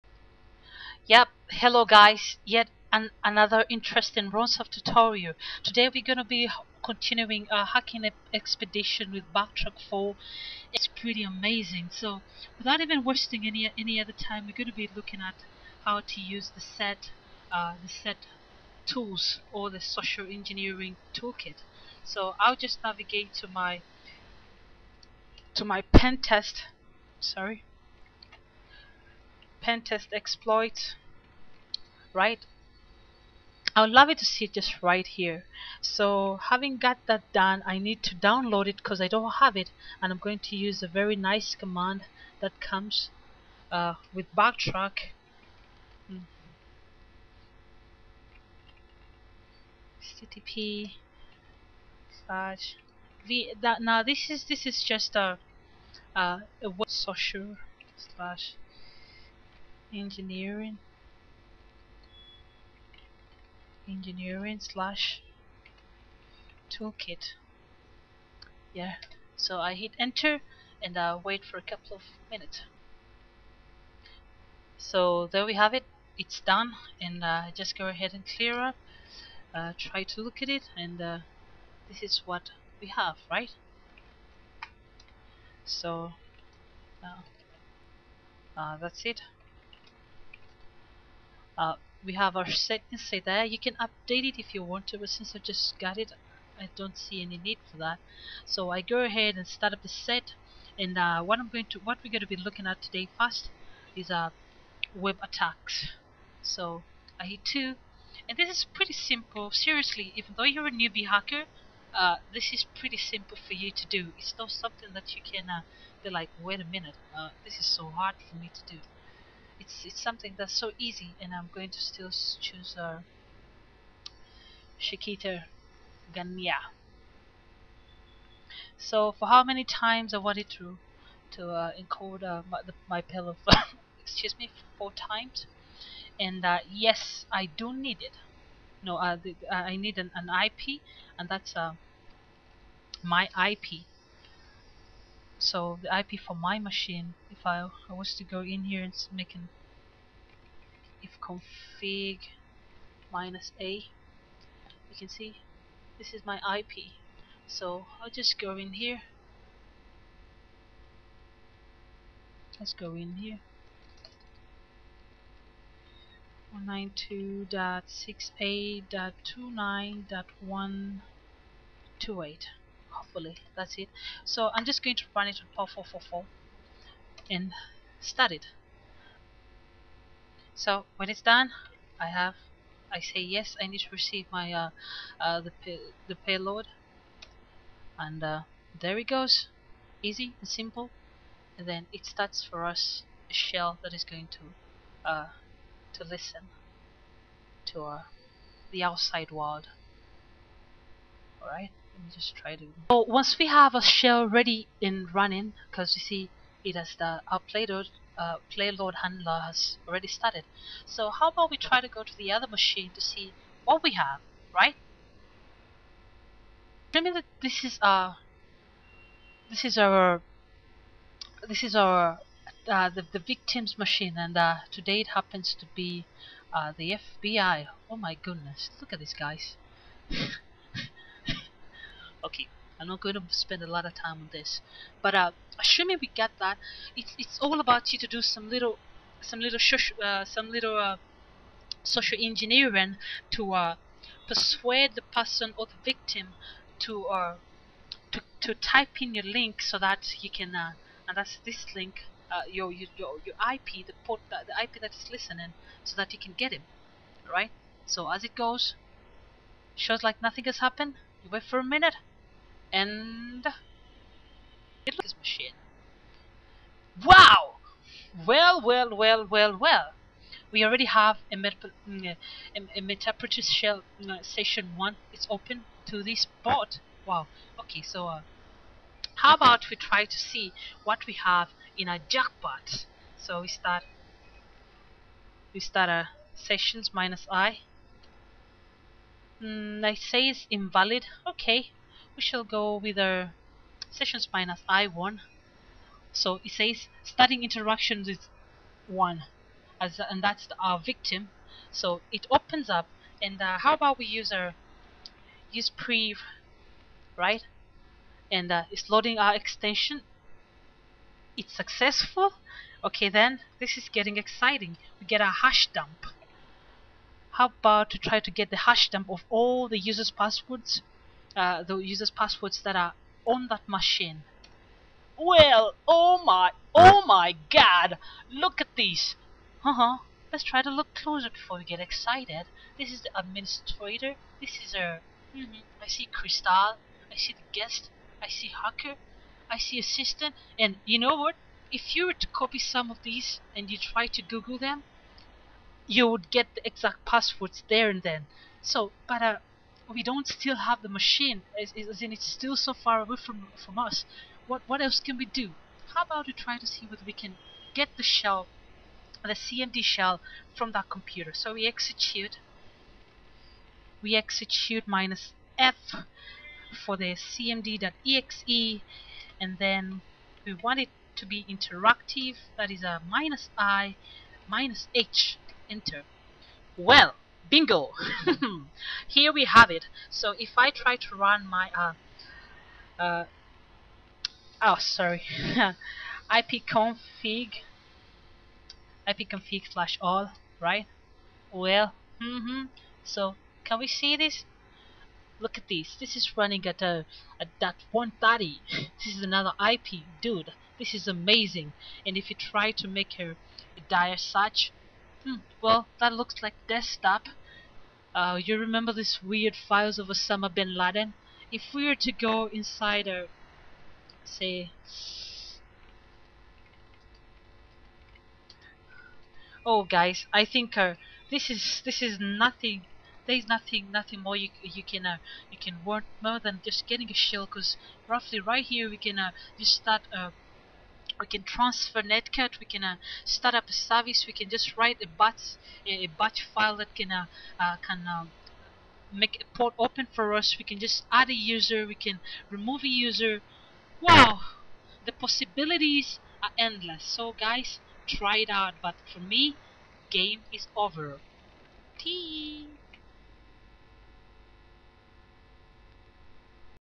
Hacking with the SET toolkit explained
This video explain the practical use of the social engineering toolkit (SET).  For more information about SET you can vistit

If anything is not so clear for you or if you want to ask me a question, please feel free to PM me.